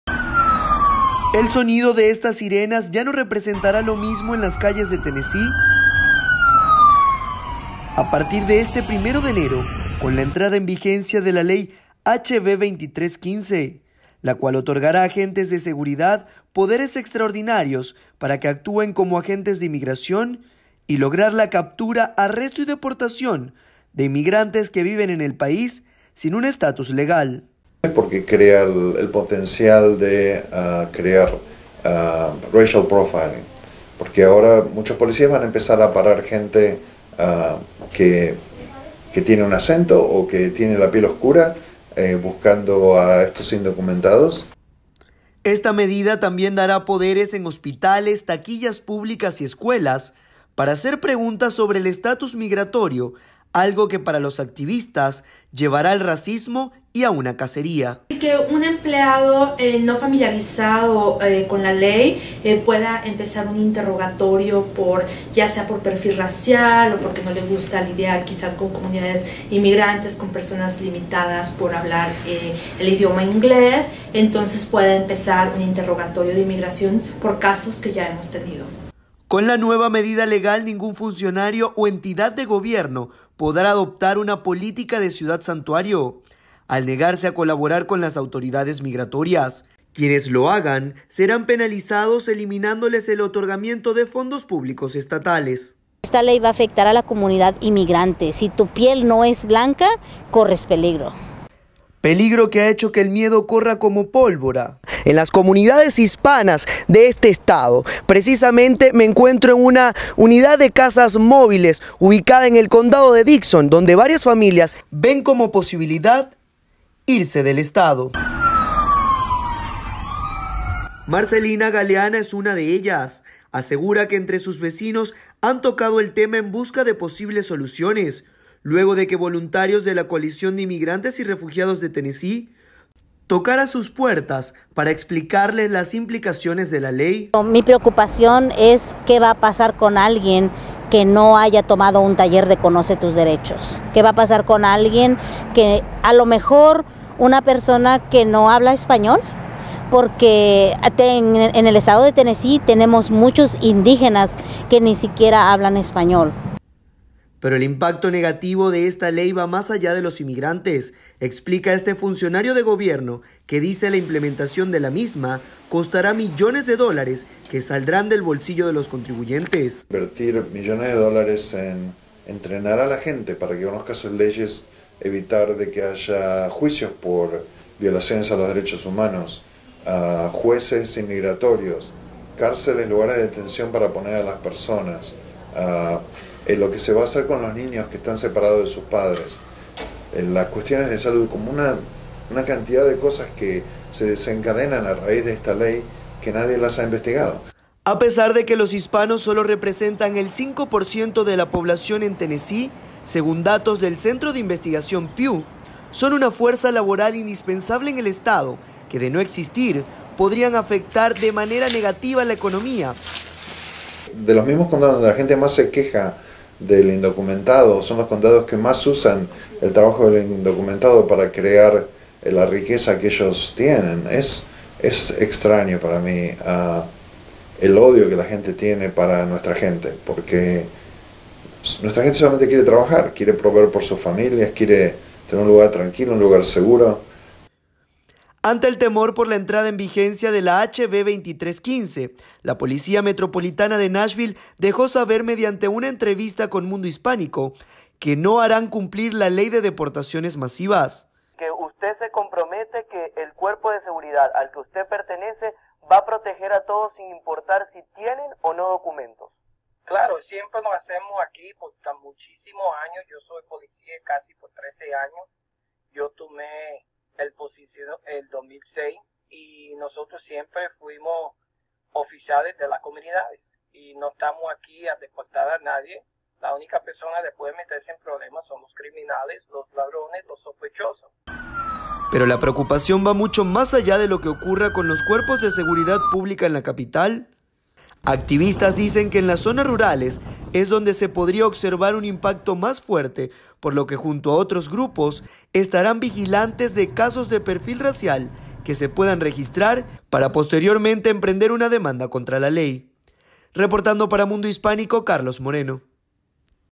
La policía podrá actuar como agentes de ICE bajo nueva ley
Una nueva ley entrara en vigencia a partir del 1 de enero, esta otorgara a agentes de policía un poder para que actúen como agentes de inmigración.